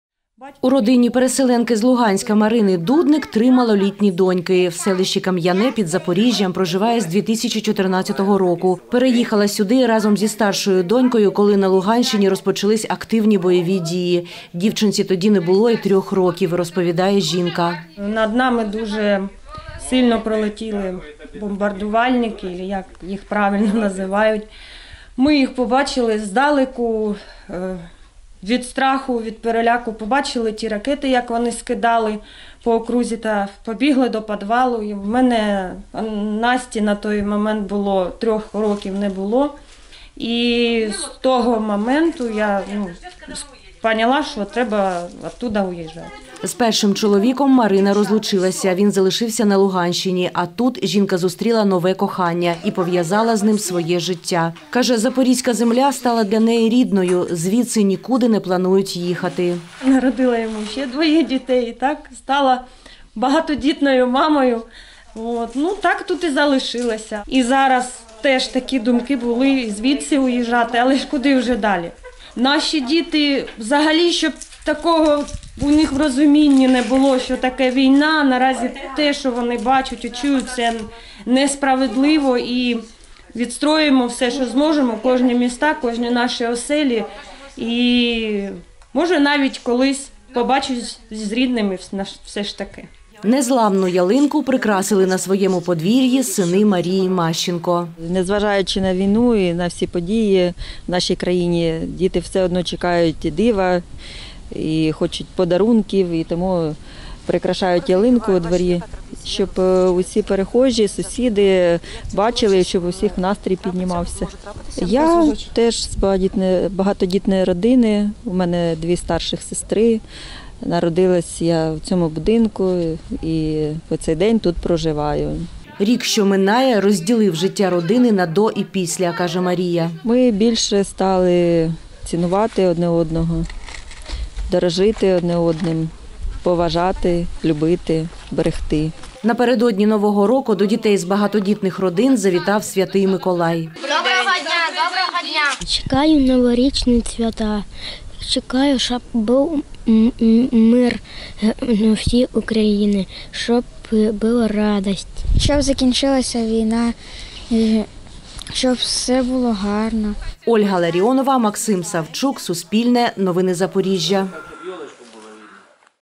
Про що мріють багатодітні родини з селища Кам’яне Запорізького району | Новини
Дві багатодітні родини проживають у селищі Кам'яне Запорізького району. Одна - переїхала з Луганська, рятуючись від обстрілів, в іншій - місцевій - з покоління в покоління виховували багато дітей. Але у новому році обидві сім’ї прагнуть одного - миру для України.